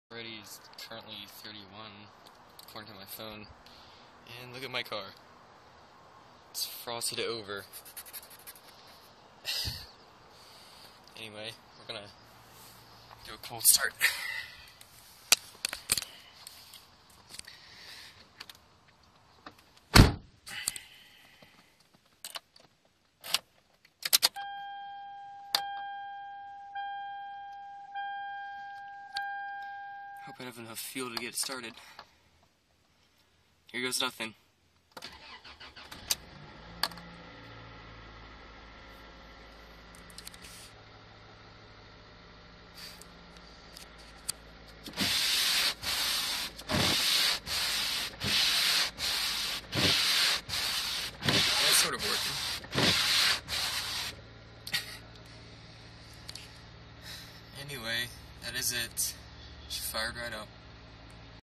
2003 Toyota Celica Cold Start 31F
(This video was taken some time ago, I have no idea when.) It was pretty cold that morning, my car was covered in frost!